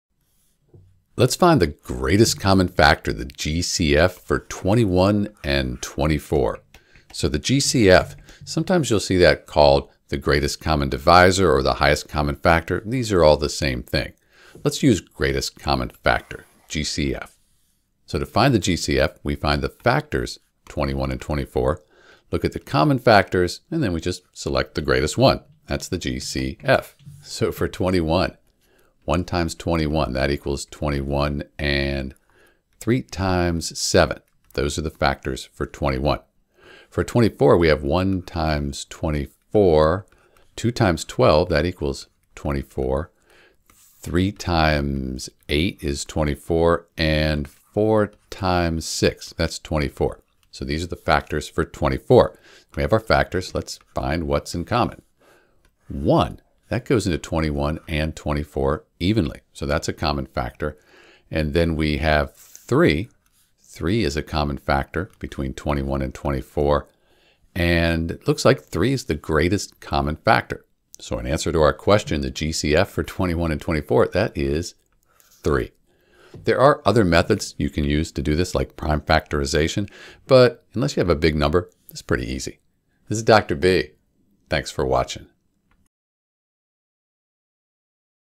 How to Find the Greatest Common Factor for 21 and 24
• The factors of 21 are the numbers that can evenly divide 21. In this case, they are 1, 3, 7, and 21.

• The factors of 24 are the numbers that can evenly divide 24. In this case, they are 1, 2, 3, 4, 6, 8, 12, and 24.

Common Factors: 1, 3

Note: You can also use prime factorization to find the GCF. It involves breaking down each number into its prime factors and then finding the common prime factors.